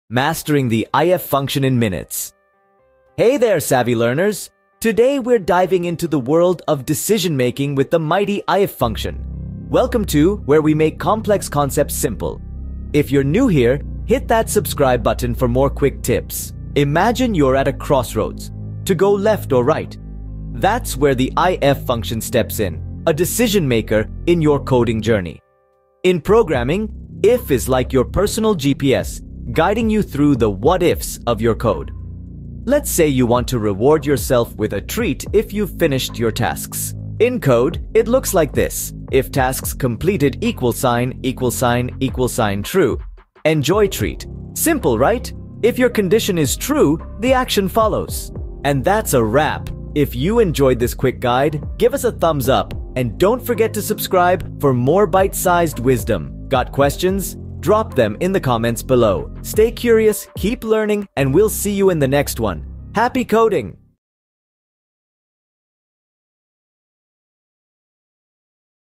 "Quick Guide: Mastering the IF Function in Minutes!"
Dive into this comprehensive guide and learn the art of Mastering the IF Function in Minutes! Whether you're a beginner or looking to brush up your Excel skills, this tutorial offers step-by-step instructions, expert tips, and secrets to help you excel in no time. Don't miss out on unlocking the power of IF functions and taking your Excel game to the next level!